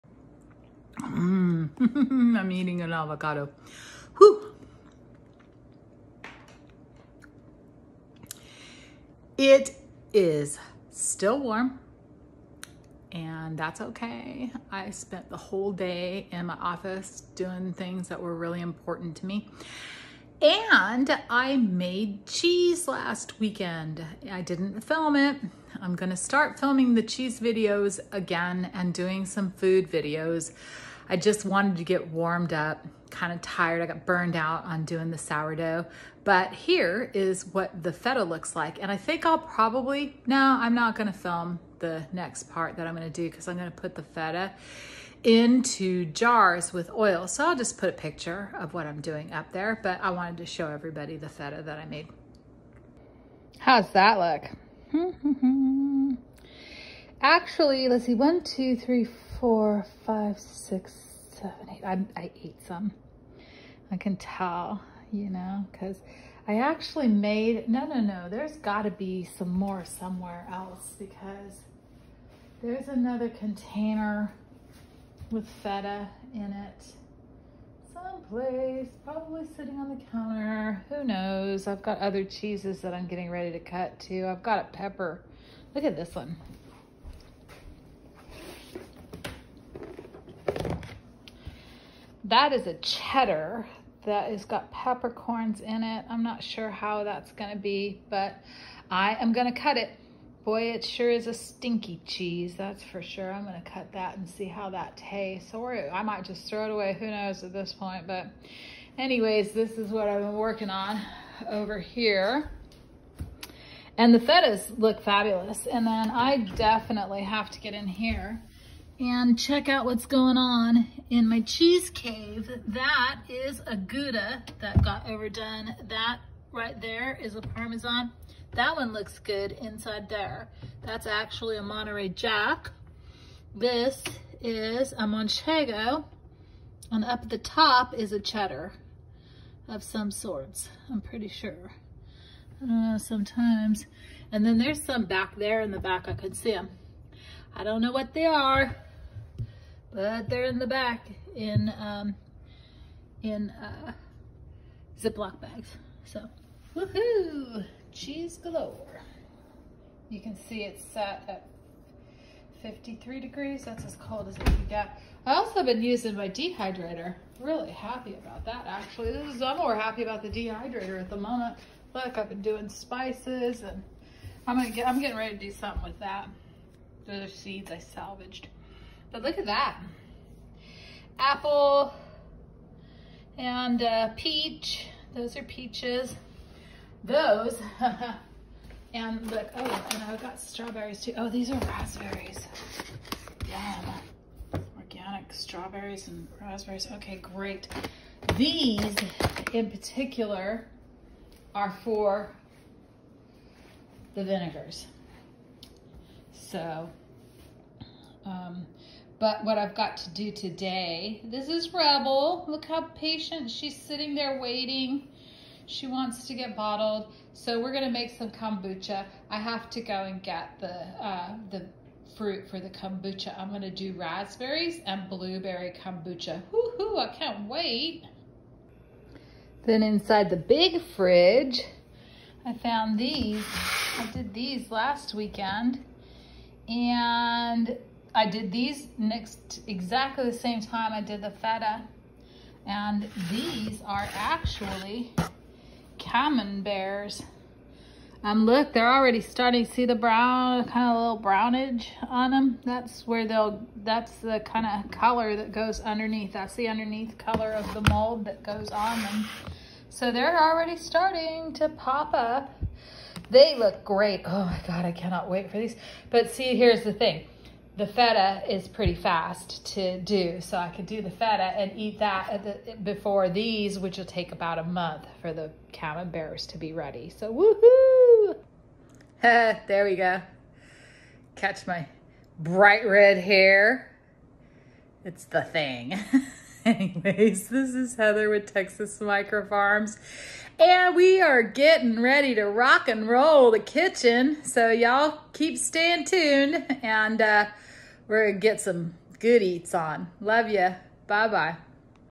lmao because I forgot to film landscape!
We are getting ready to relaunch the Texas Micro Farms cooking videos.  Soooooo this video is a quick video to get myself ready for filming again.

So thanks for putting up with this Sexy Beast while ahe works it out!